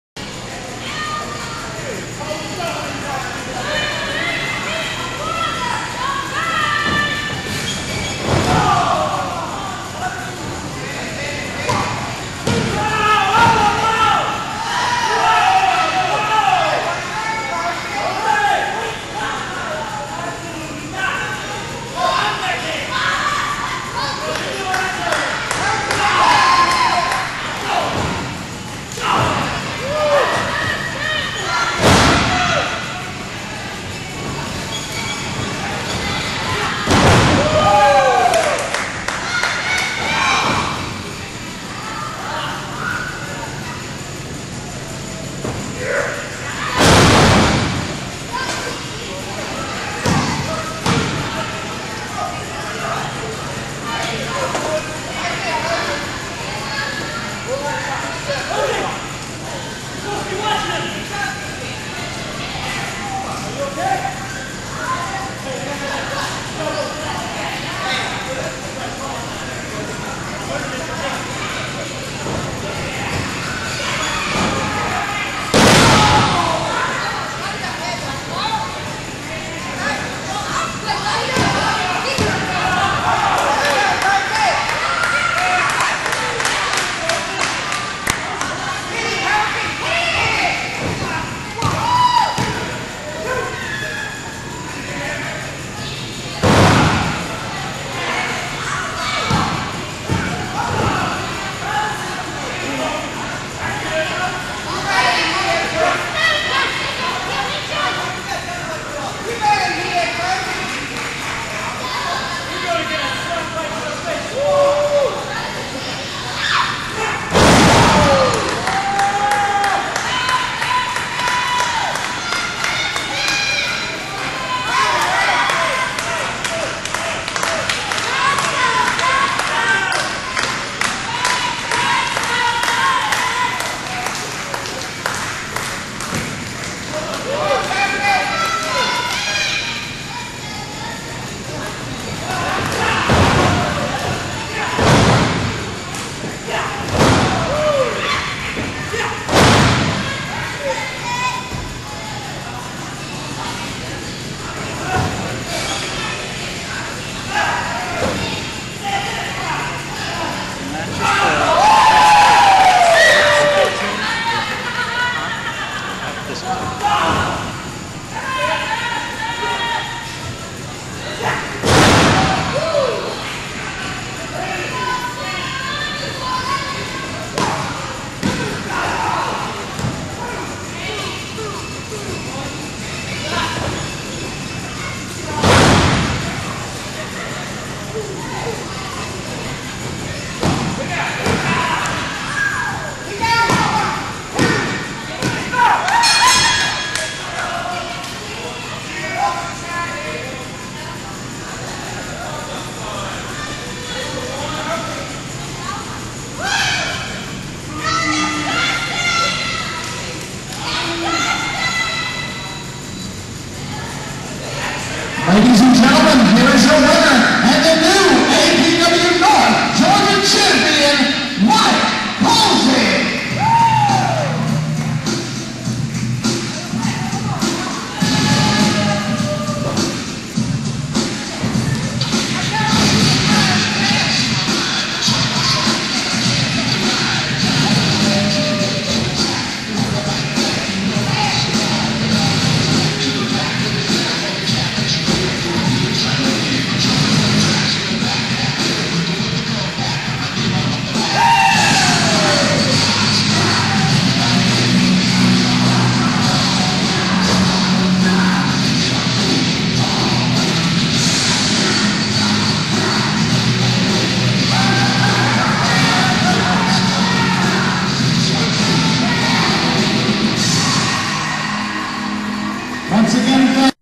Mike Posey vs Seth Delay vs Dustin Knight APW 7-3-09 PT 2
Mike Posey vs Seth Delay vs Dustin Knight
North Georgia Heavyweight Title Match
APW Royston, GA 7--09 PT 2
Referee is Dee Byers